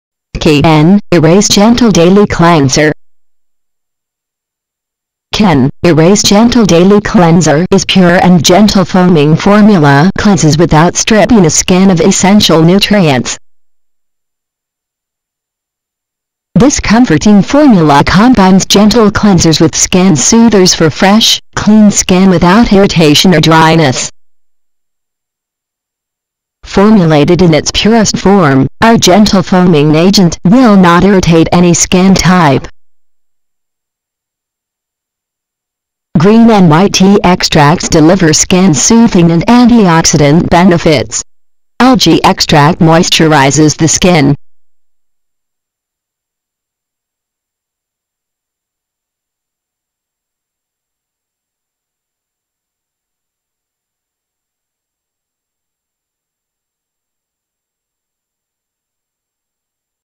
Kinerase Gentle Cleanser
Kinerase Gentle Daily Cleanser - A pure and gentle foaming formula with green tea extract for additional skin soothing benefits. Kinerase Gentle Daily Cleanser is ideal for even the most sensitive skin types.